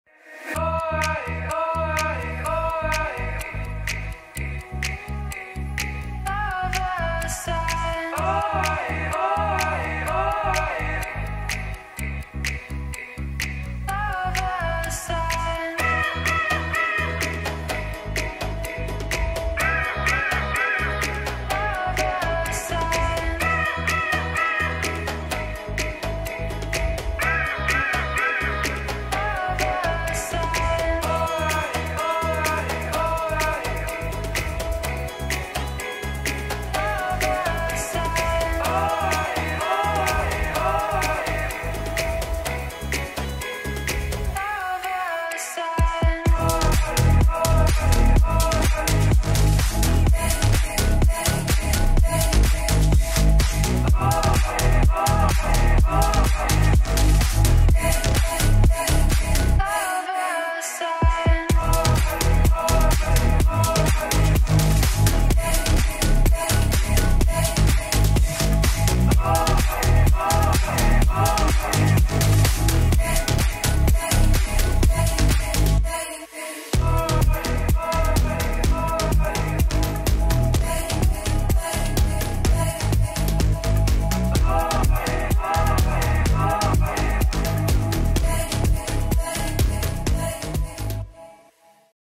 [Vatsim] [EDDB] Berlin Real Ops - Morning Edition 2023 timelapse
Here is a timelapse of the "Berlin Real Ops - Morning Edition" event which took place on the 12th of February 2023 between 0500z and 0900z on the Vatsim network.

63 departures and 26 arrivals were registered during this event.